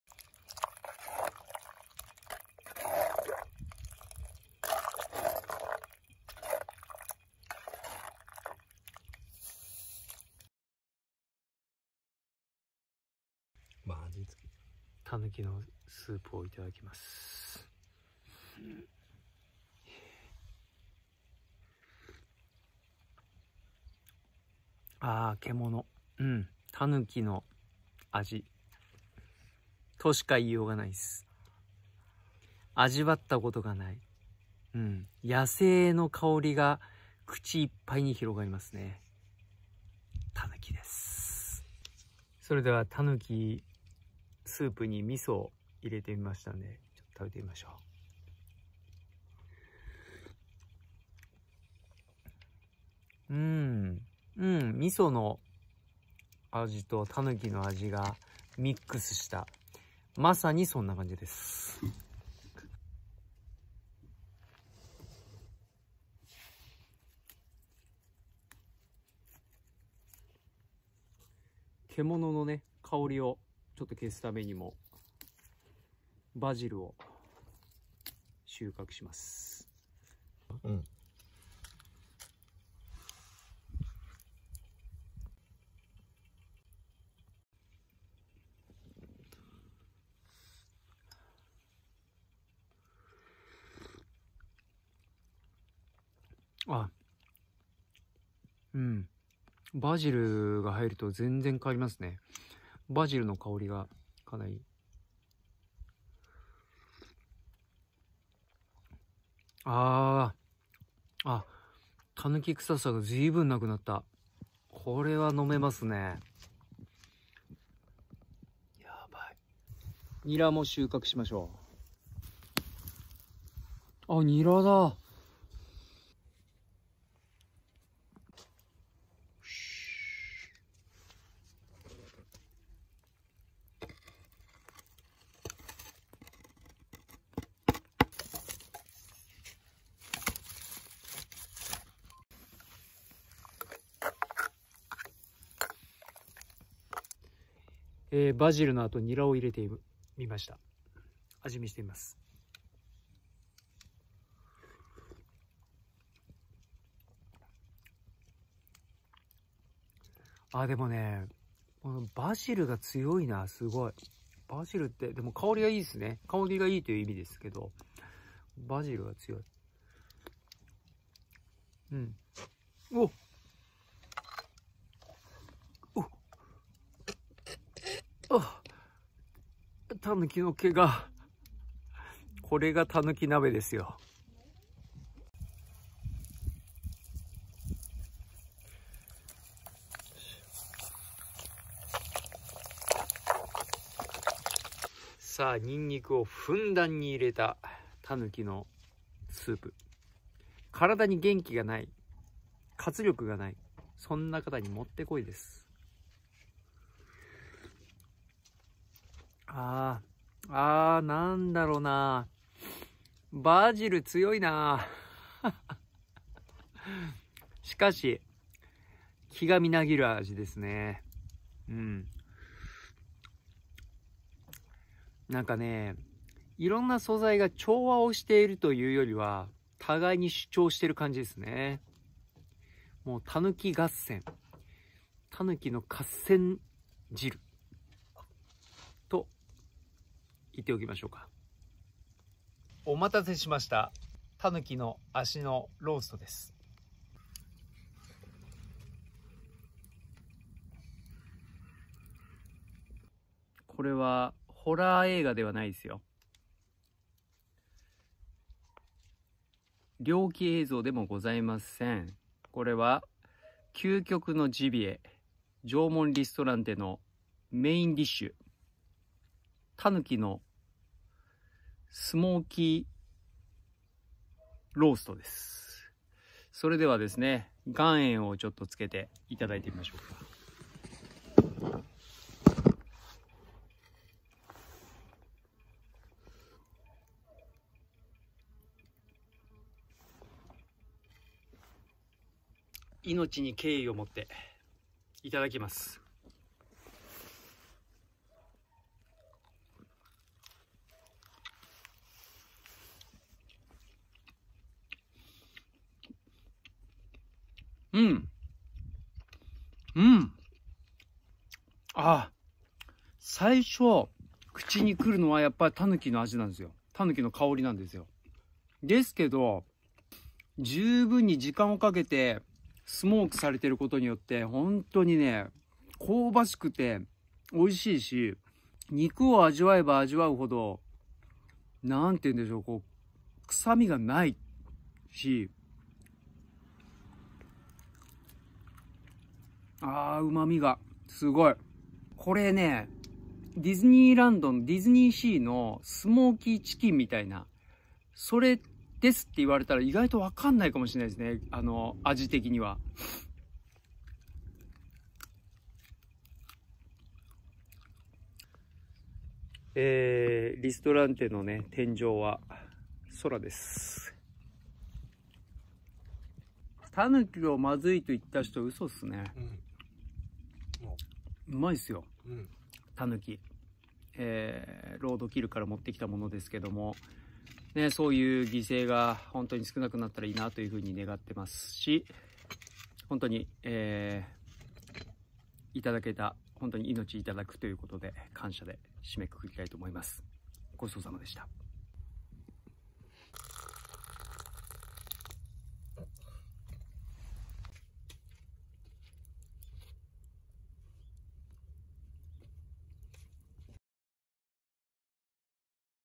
【縄文小屋編】たぬきの皮なめし〜中編〜《食べるシーンのみ》
たぬきの皮なめしシリーズ中編はなめしシーンはお休みで食べるシーンのみです。
タヌキ汁とスモーキーローストのお味はいかに…